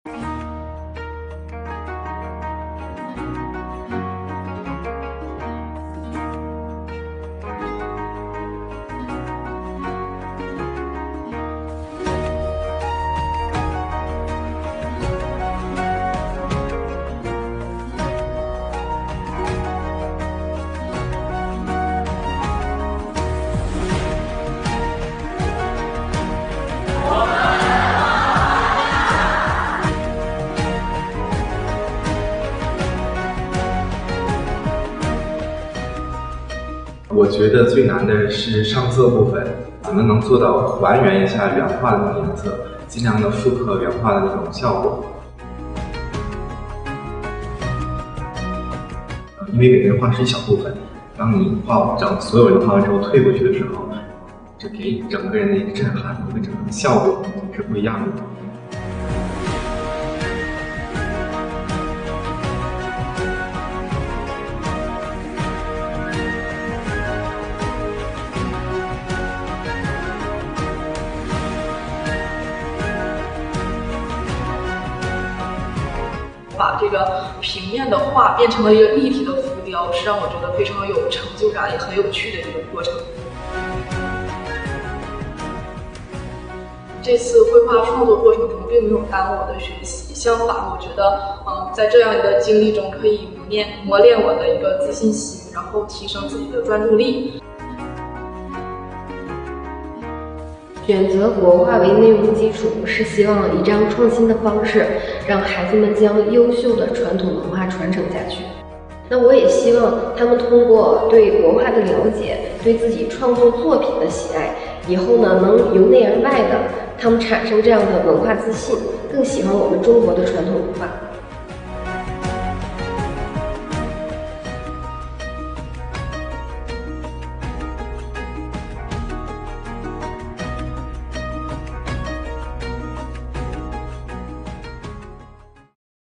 【21名高中生把教室畫成千裏江山圖】近日，長春市21名高中生以教室墻壁為畫布，重繪《千裏江山圖》。完成後的《千裏江山圖》浮雕壁畫高近3.5米，總長度11米，青翠欲滴，氣勢恢宏。
近日，長春市21名高中生以教室墻壁為畫布，重繪《千裏江山圖》。完成後的《千裏江山圖》浮雕壁畫高近3.5米，總長度11米，青翠欲滴，氣勢恢宏。